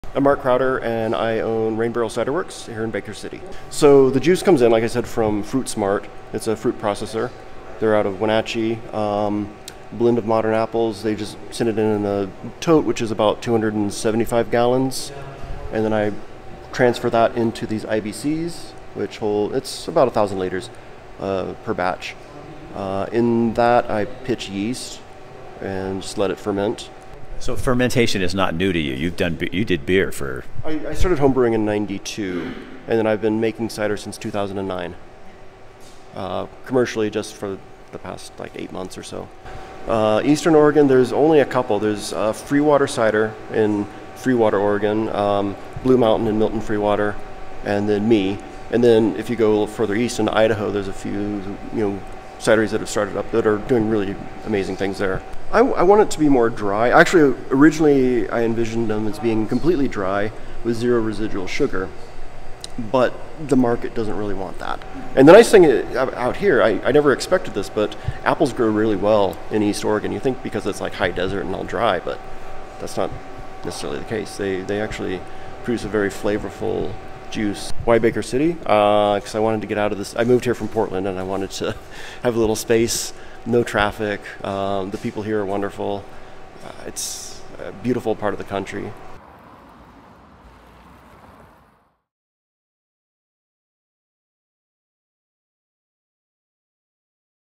Cidermaker In Baker City
Fermentation is a way of life in Baker County. Beer's been a big deal for a while and spirits are moving into the area as well. So it should surprise no one that cider is pressing it's way into the market.